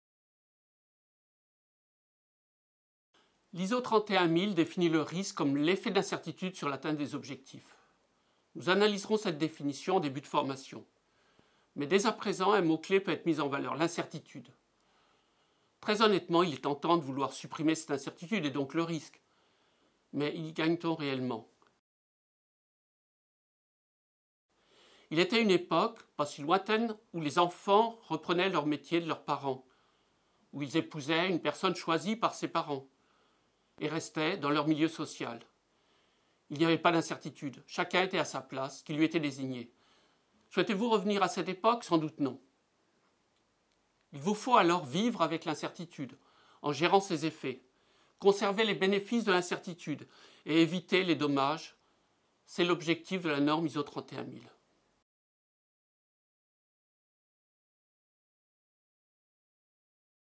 2-ISO 31000-English-Risk Management
INSA offers a free online training course on ISO 31000 "Risk management - Guidelines". To take this course, log onto
and select the "Safety Management" Module and then "ISO 31000. Risk management (English version)".
SEAMOnLine is associated with the face-to-face Advanced Master's degree "Safety Engineering and Management" (SEAM):